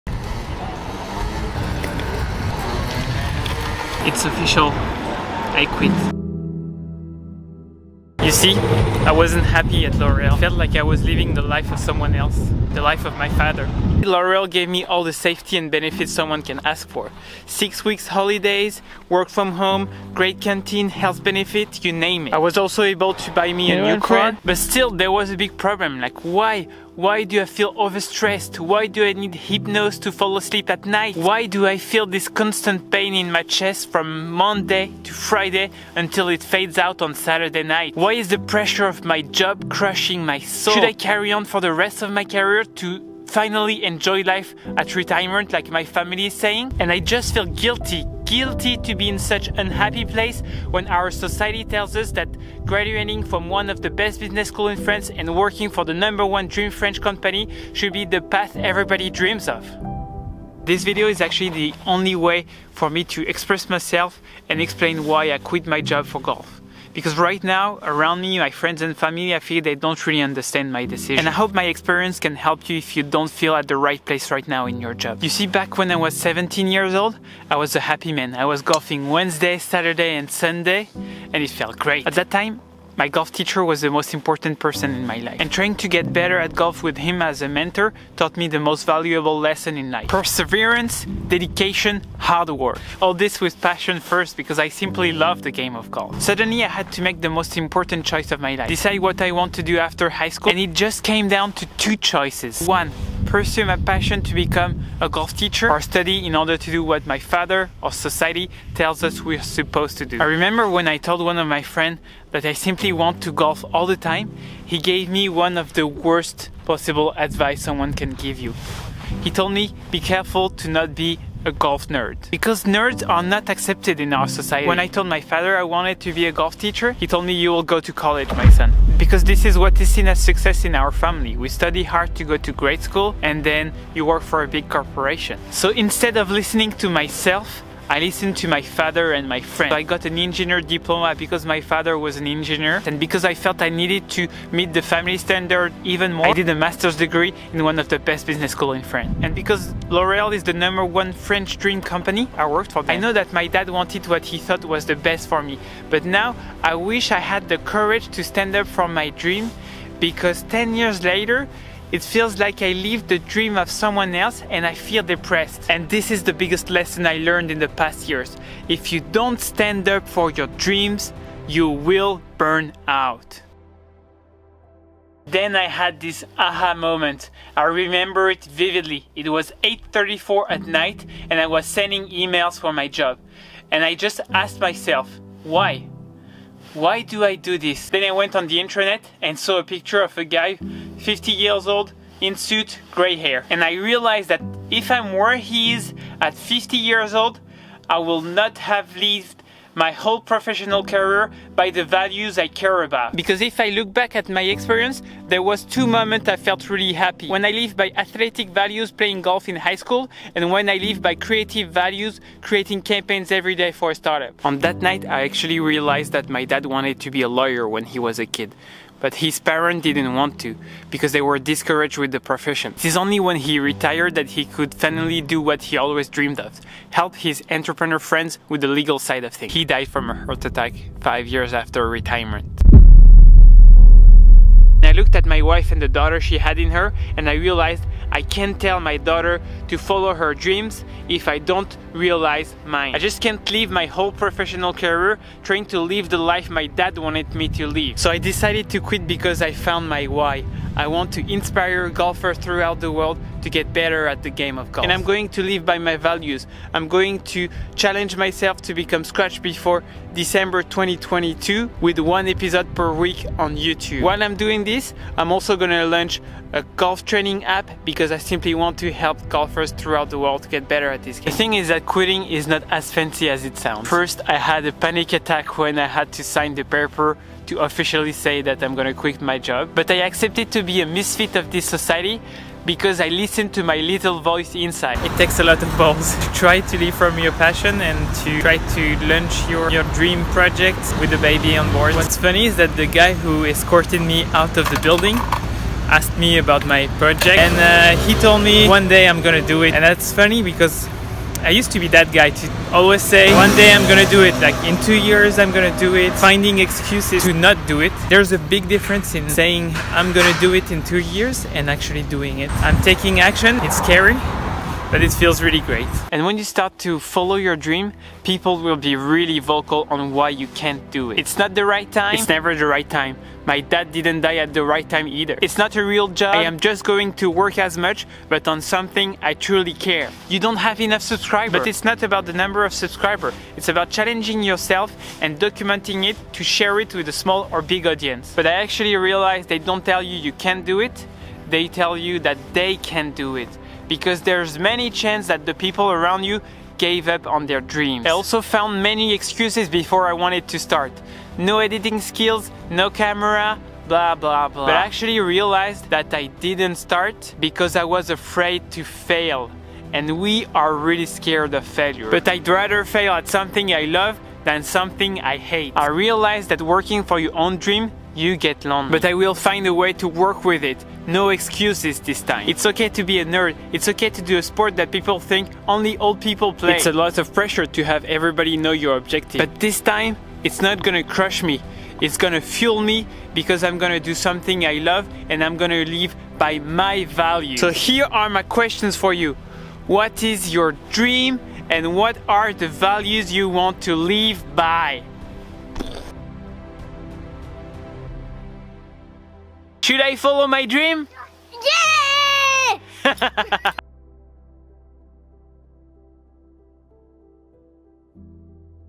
I quit my job for golf after learning 3 life lessons
I quit my job for my dream after learning these 3 life lessons.
This video is actually the only way for me to express myself on why I quit my job for golf because my family and friend don't understand my decision right now.
And I hope my experience will help you if you don't feel at right place right now in your job.
I hope this video will inspire you to follow your dream and live by your values.